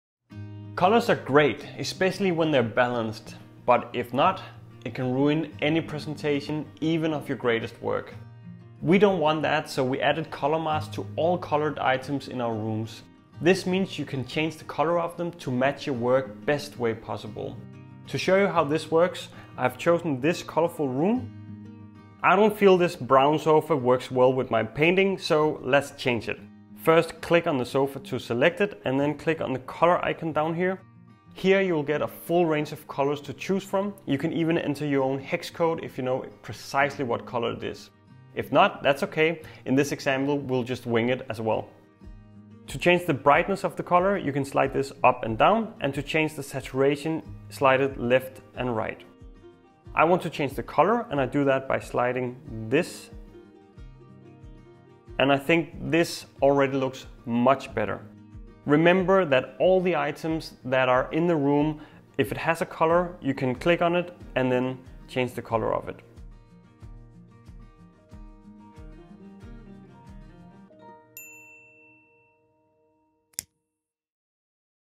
How to change color of items in Canvy
Keeping a color theme that matches your style is important. You can change the color of all our colored items so they match your style and don’t clash with it.

Try it now – works in your browser, on your phone, tablet or desktop 👍 Just open your favorite browser and go

Ps.